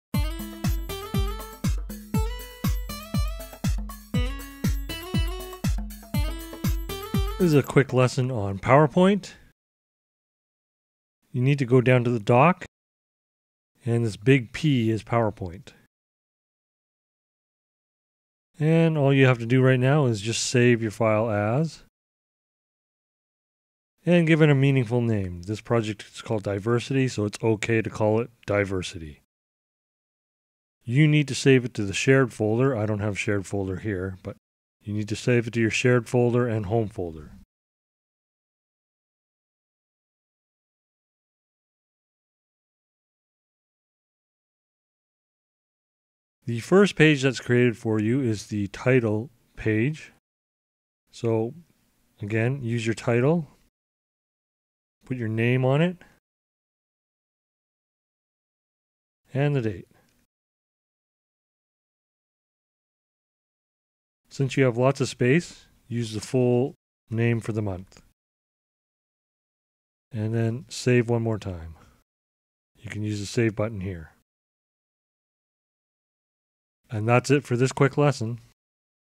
PowerPoint Meaningful Filename and Title
Mini lesson on saving files and changing file names and creating a PowerPoint file.